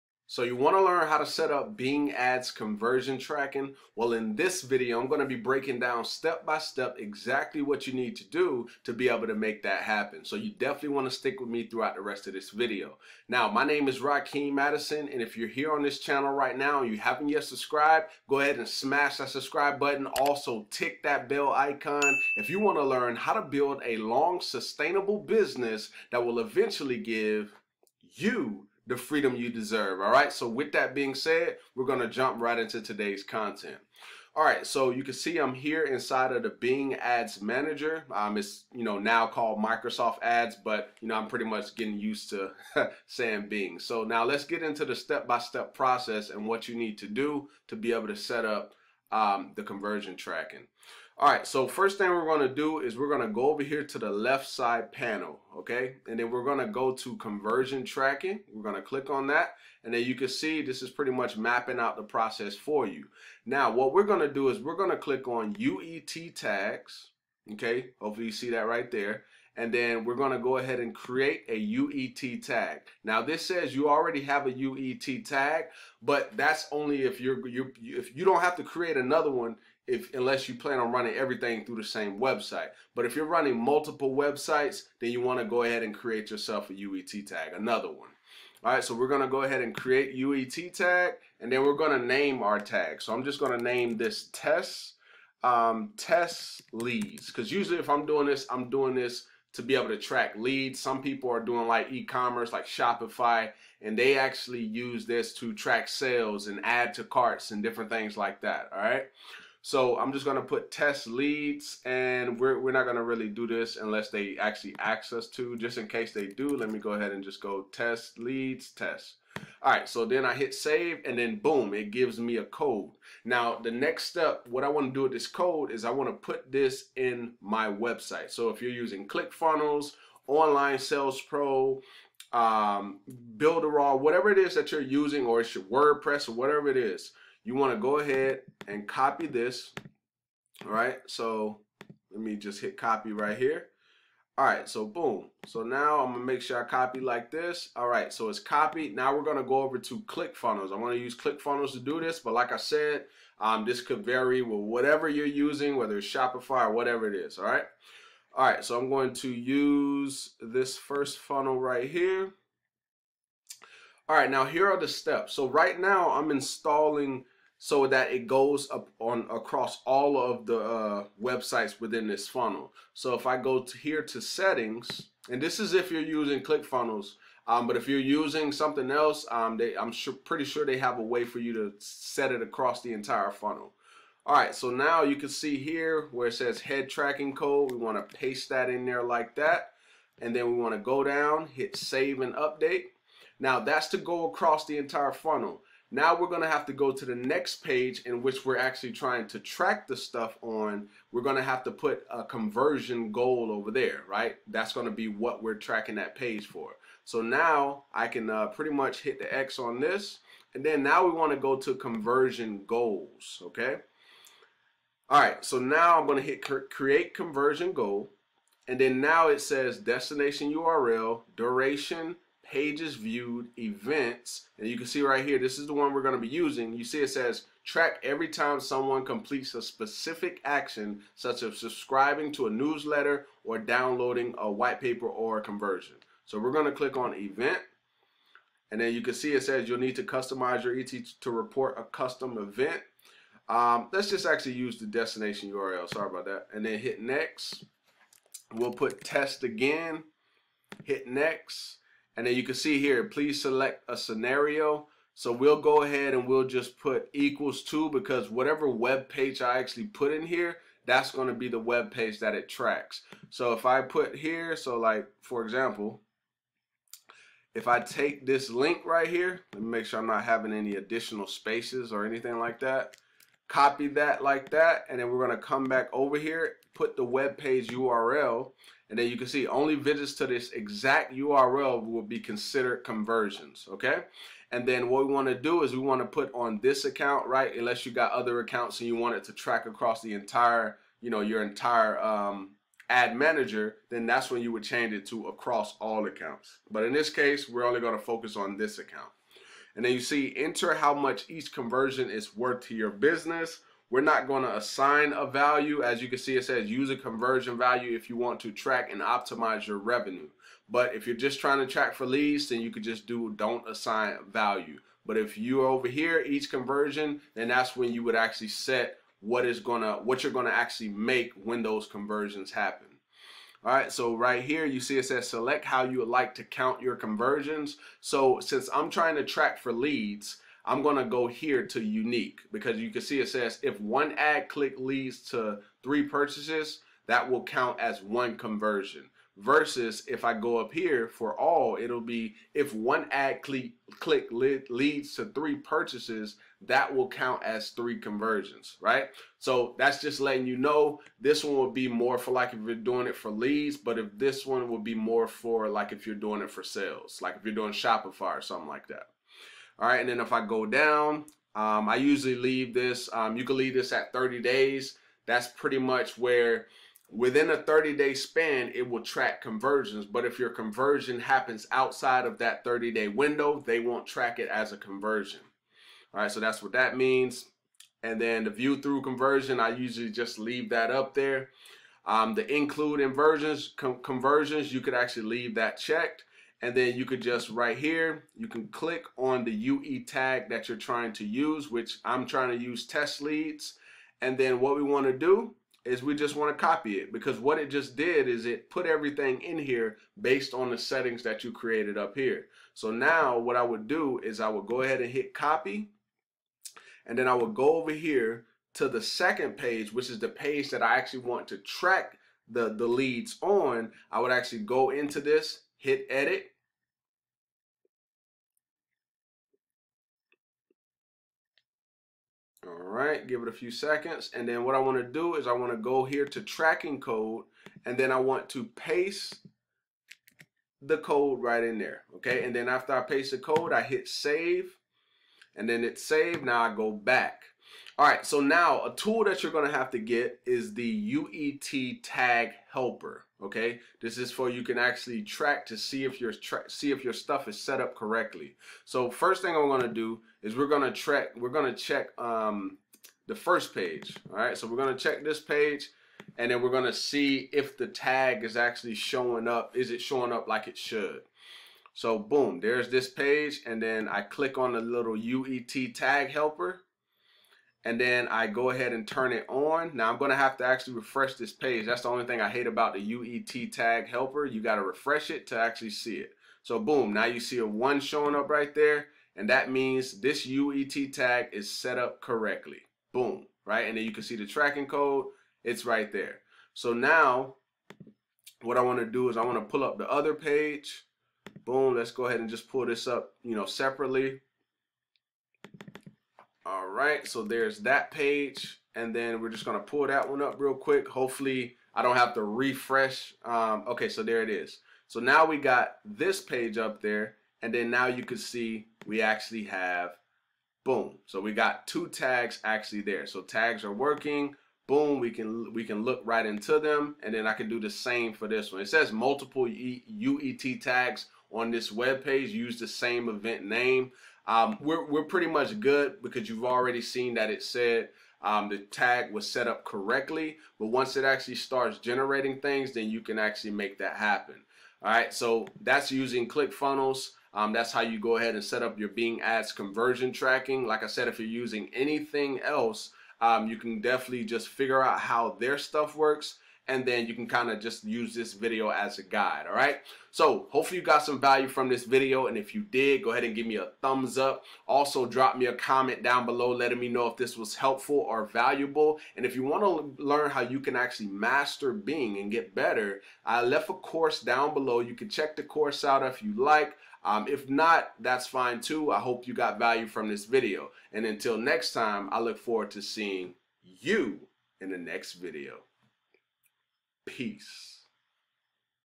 Bing Ads Conversion Tracking - SIMPLE Setup Process To Track Conversions
*** Want a 1 on 1 Coaching Call For Bing Ads ? MESSAGE ME HERE ➡️➡️➡️

In this video I go over how to setup Bing Ads Conversion Tracking in 2020 and I will show you different ways you can track conversions in Bing Ads so prepare to learn about tracking website conversions and helping you understand the performance of your campaigns

I go over Bing Ads Conversion Setup from start to finish, so you can not only create important actions for your website but you can also optimize for them in your campaigns.